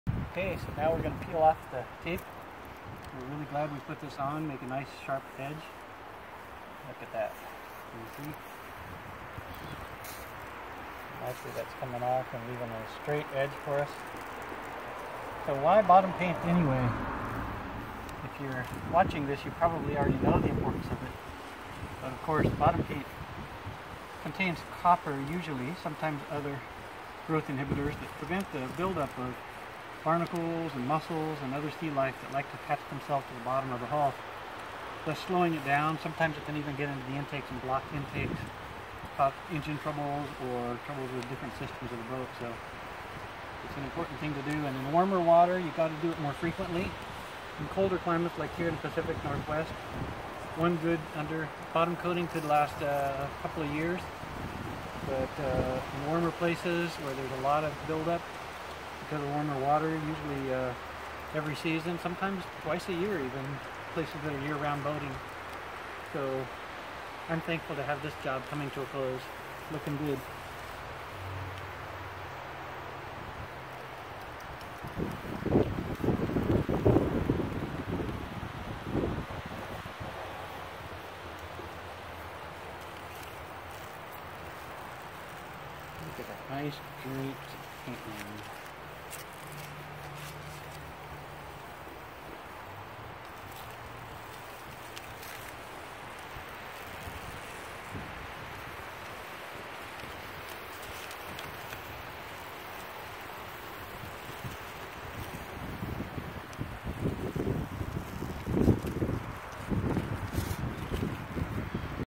Captain Randy: How to Bottom Paint a Boat: Part 6
Randy explains why and how often this procedure is required on the Oregon Coast